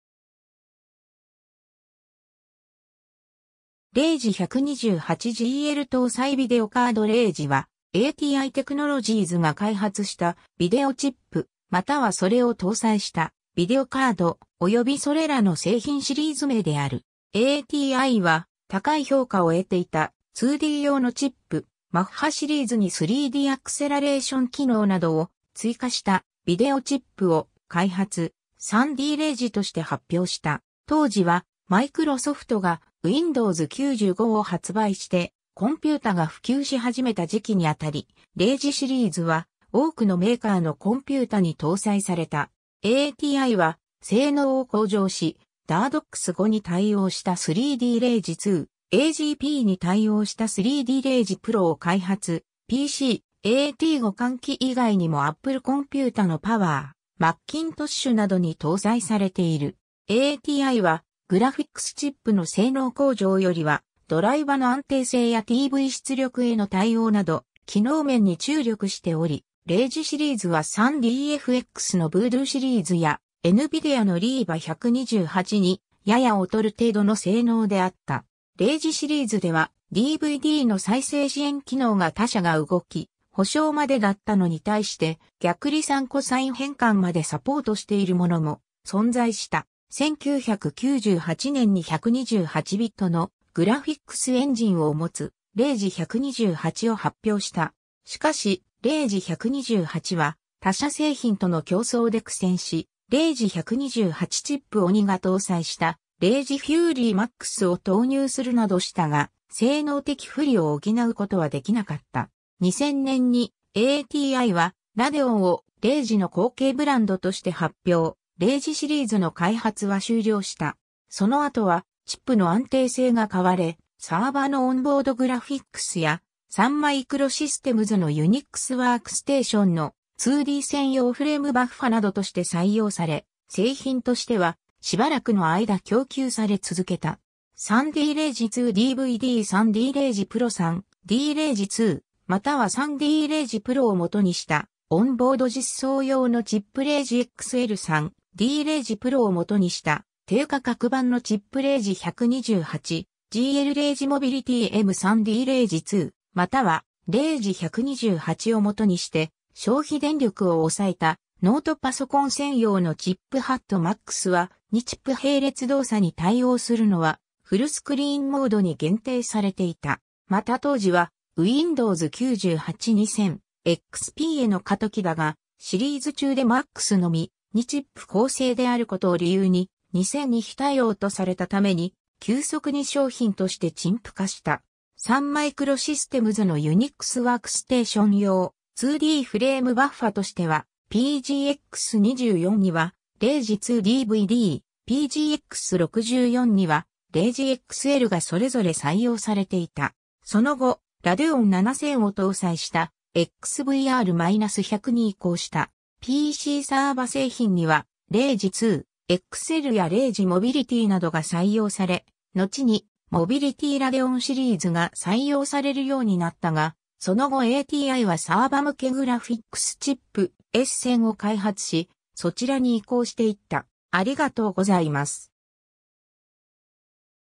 ATI Rage
RAGE 128 GL搭載ビデオカード RAGE（レイジ）は、ATI Technologies（現AMDのグラフィックスアクセラレータ開発部門）が開発したビデオチップ（グラフィックスアクセラレータ）、またはそれを搭載したビデオカード、およびそれらの製品シリーズ名である。
 ATIは、高い評価を得ていた2D用のチップ、Machシリーズに3Dアクセラレーション機能などを追加したビデオチップを開発、3D RAGEとして発表した。
当時はマイクロソフトがWindows 95を発売してコンピュータが普及し始めた時期にあたり、RAGEシリーズは多くのメーカーのコンピュータに搭載された。
ATIは、性能を向上しDirectX5に対応した3D RAGE II、AGPに対応した3D RAGE Proを開発、PC/AT互換機以外にもアップルコンピュータのPower Macintoshなどに搭載されている。
ATIは、グラフィックスチップの性能向上よりは、ドライバの安定性やTV出力への対応など機能面に注力しており、RAGEシリーズは3dfx（後にNVIDIAに買収される）のVoodooシリーズやNVIDIAのRIVA 128にやや劣る程度の性能であった。
RAGEシリーズではDVDの再生支援機能が他社が動き補償(MC)までだったのに対して逆離散コサイン変換までサポートしているものも存在した。
 1998年に128ビットのグラフィックスエンジンをもつRAGE 128を発表した。
しかしRAGE 128は他社製品との競争で苦戦し、RAGE 128チップを2ヶ搭載したRAGE FURY MAXXを投入するなどしたが、性能的不利を補うことは出来なかった。
 2000年にATIは、RADEONをRAGEの後継ブランドとして発表、RAGEシリーズの開発は終了した。
 その後はチップの安定性が買われ、サーバのオンボードグラフィックスや、サン・マイクロシステムズのUNIXワークステーションの2D専用フレームバッファなどとして採用され、製品としてはしばらくの間供給され続けた。
 3D RAGE II +DVD 3D RAGE Pro 3D RAGE IIまたは3D RAGE Proを元にしたオンボード実装用のチップ RAGE XL 3D RAGE Proを元にした低価格版のチップ RAGE 128 GL RAGE Mobility-M 3D RAGE IIまたはRAGE 128を元にして消費電力を抑えたノートパソコン専用のチップ ^ MAXXは、2チップ並列動作に対応するのは、フルスクリーンモードに限定されていた。
また当時は、Windows98→2000/XPへの過渡期だが、シリーズ中でMAXXのみ、2チップ構成であることを理由に、2000に非対応とされた為に、急速に商品として陳腐化した（後に、対応ドライバはリリースされた）。
 ^ サン・マイクロシステムズのUNIXワークステーション用2Dフレームバッファとしては、PGX24にはRAGE II+DVD、PGX64にはRAGE XLがそれぞれ採用されていた。
その後、Radeon7000を搭載したXVR-100に移行した。
PCサーバ製品にはRAGE II/XLやRAGE Mobilityなどが採用され、のちにMobility RADEONシリーズが採用されるようになったが、その後ATIはサーバ向けグラフィックスチップ「es1000」を開発し、そちらに移行していった。